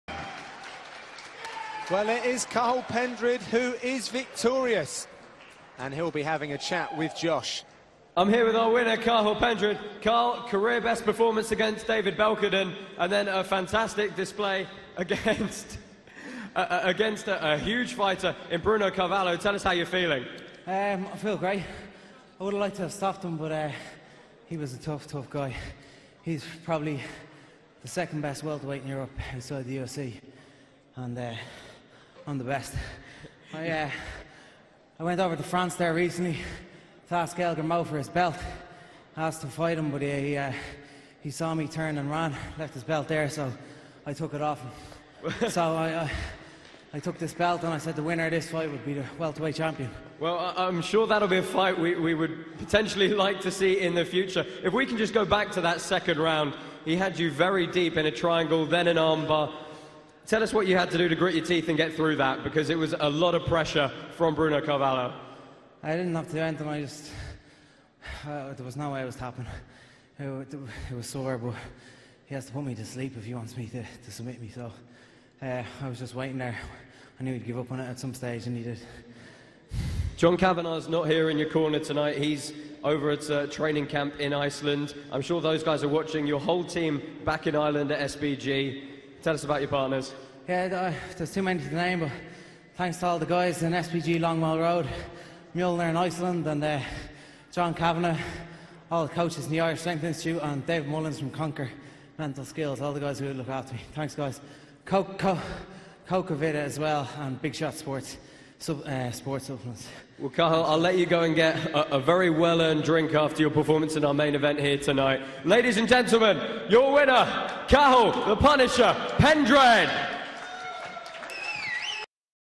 Cathal Pendred calls out Gael Grimaud in the cage following win over Carvalho [SevereMMA.com]
Cathal "The Punisher" Pendred calls out Cage Warriors Welterweight Champion Gael Grimaud in the cage following win over Bruno Carvalho in Cardiff, Wales